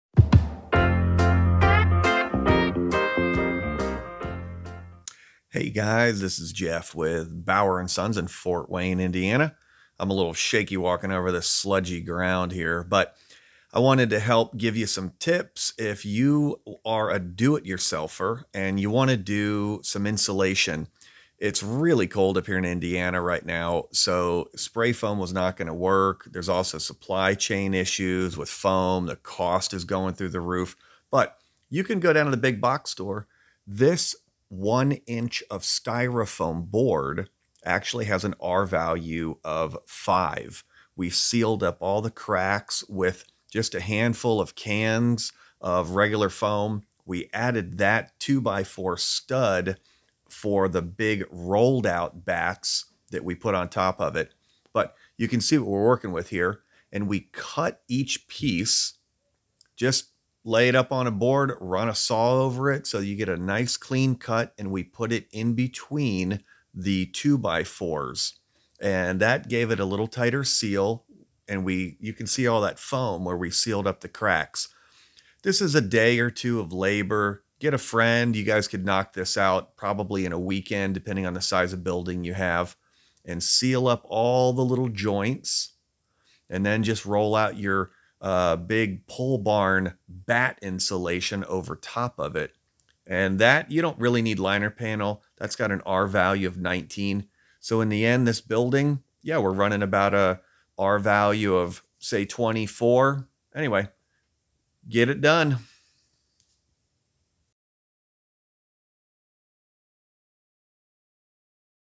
Easy Barn Insulation
Jeff with @barnandhome  gives a really quick walk-through on how to get your own barn insulated.

Due to supply issues and trying to spray foam in cold temps, we opted to use a 1" foam board and batt insulation to create an R24 value in this new storage shop.

The ceiling will eventually get liner panel and blown cellulose with an R value of 40.

White foam board - or any other color really, is easy to find, and you can cut it to fit in between the wall purlins.  We then used cans of spray foam to seal each joint and sprayed behind every post and in the corners of the building.  This was to create as best of an air-tight seal as we could.  The barn already had house wrap.

This 30x40x10 building was fully insulated with the foam board and rolls of 4' wide batt insulation by a 5 man crew in 6 hours.